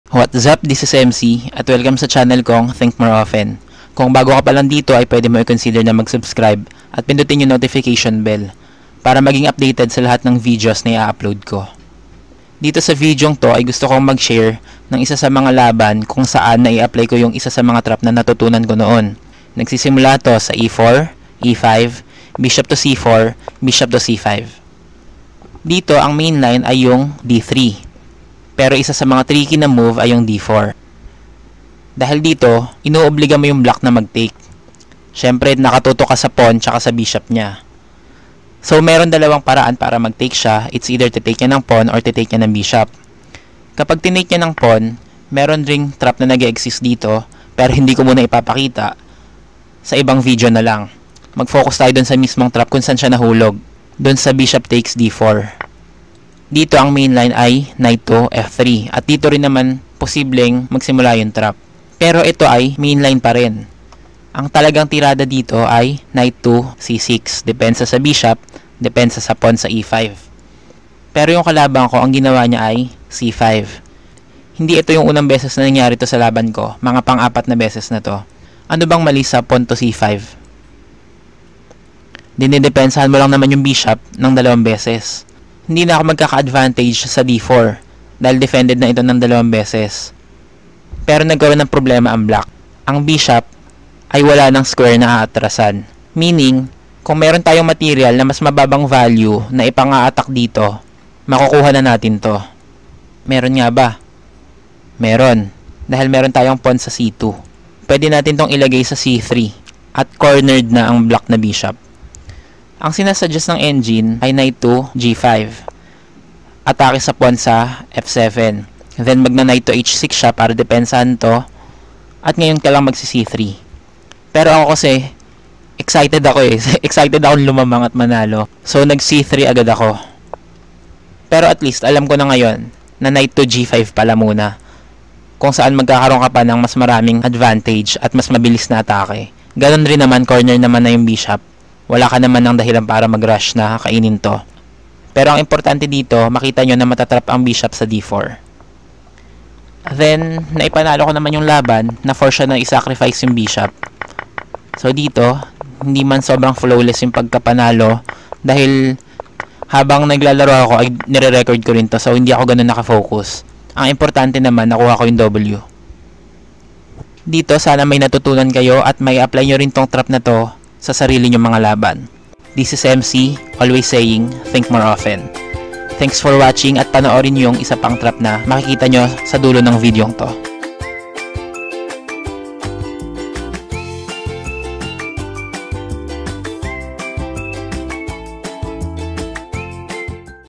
One of the traps in Bishop's Opening Basic Chess Traps You Must Know
This is one of the traps I've never read or watched but I figured out by myself. I believe the reason is it is too easy to find and no good players will fall for it.

Mark Christian Evangelista

Trap/Patibong sa Scandinavian!